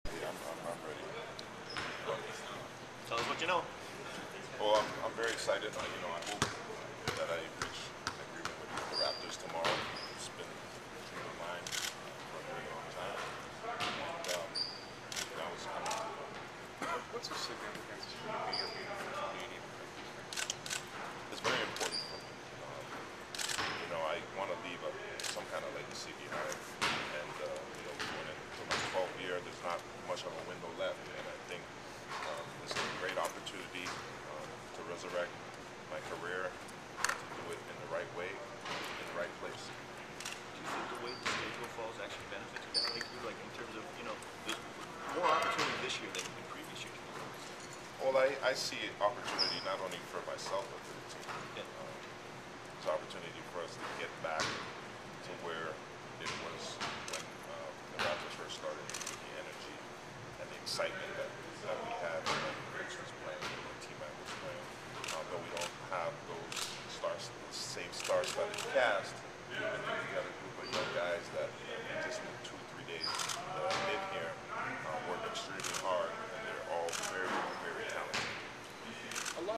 Magloire Media Scrum
Here's Toronto native and free agent Jamaal Magloire speaking with the media after informal workouts at ACC.